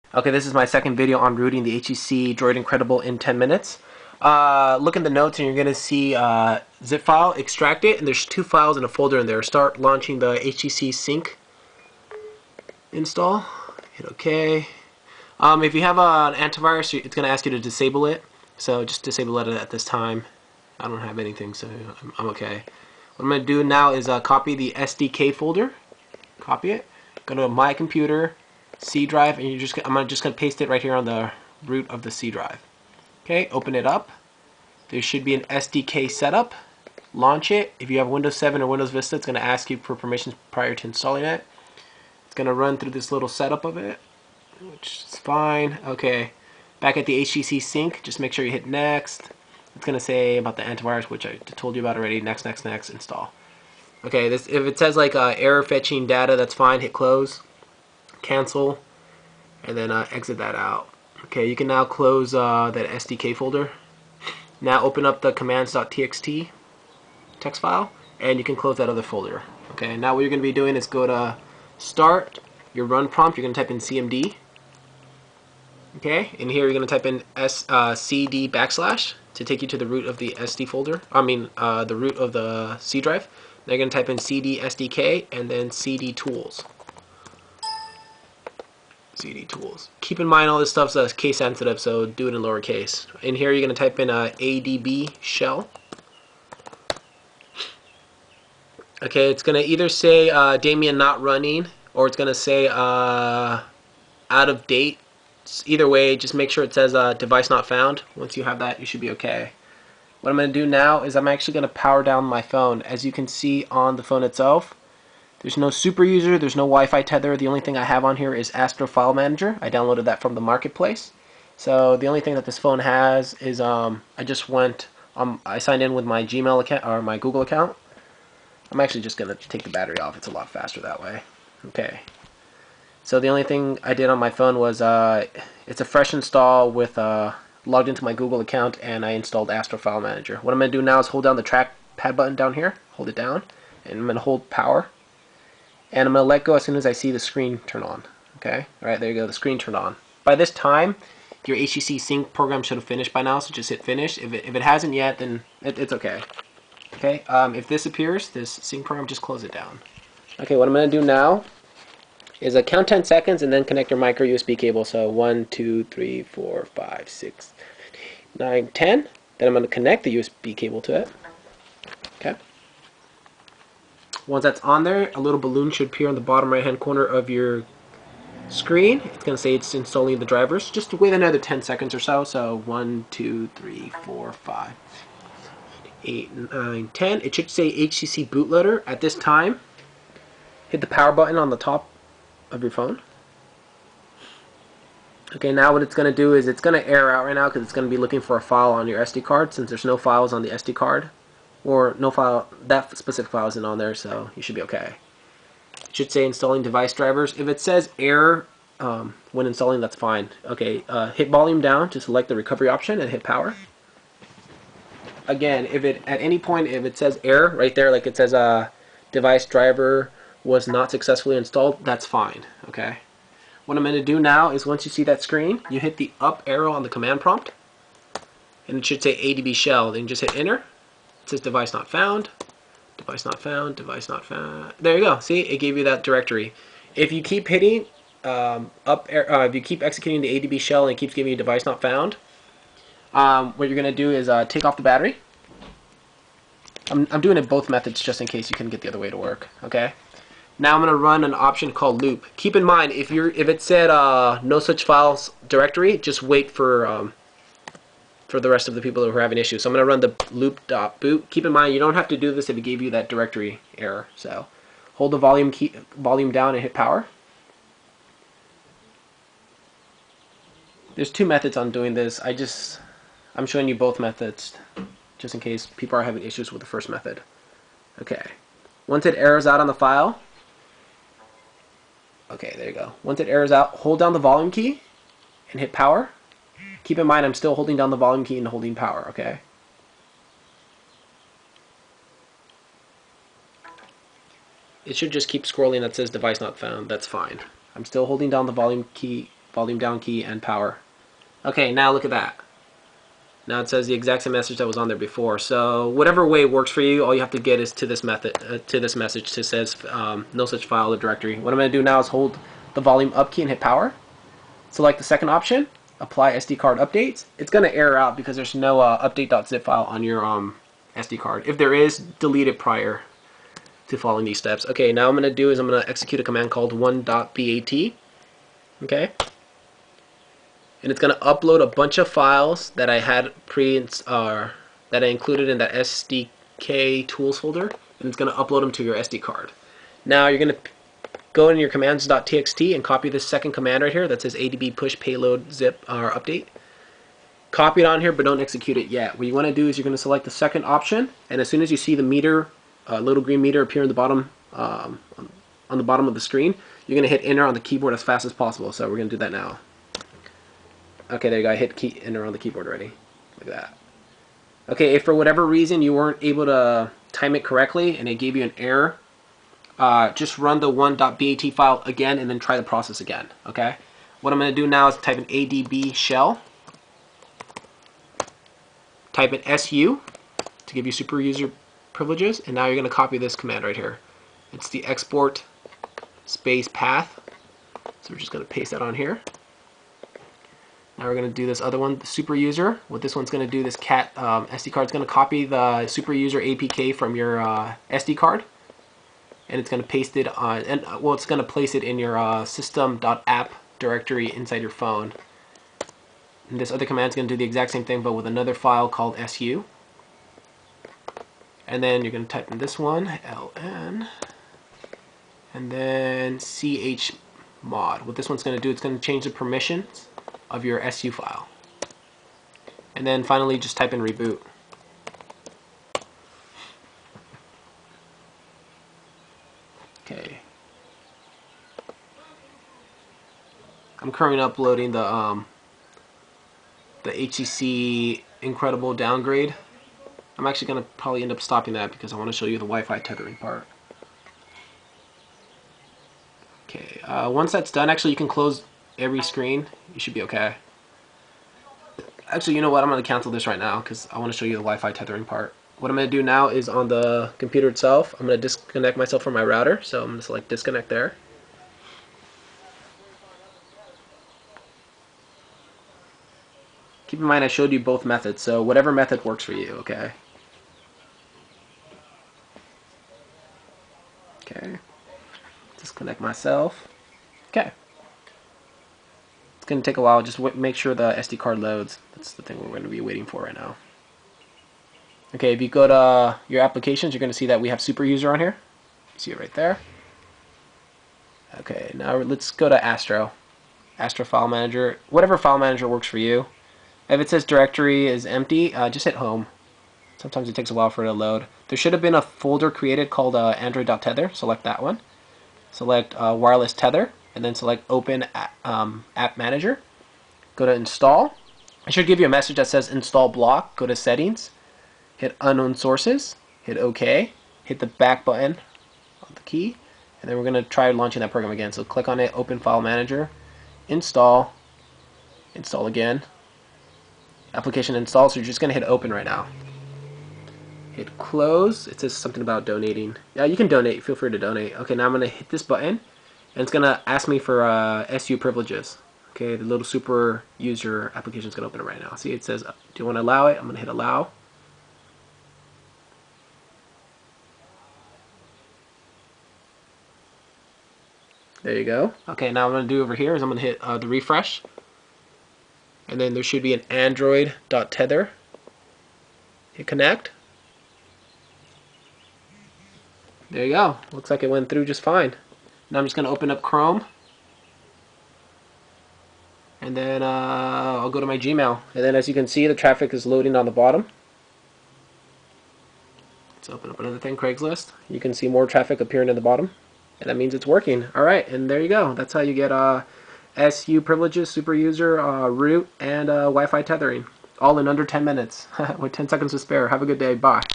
Droid Incredible Root in 10mins
Rooted, installed superuser and wifitether working in 10mins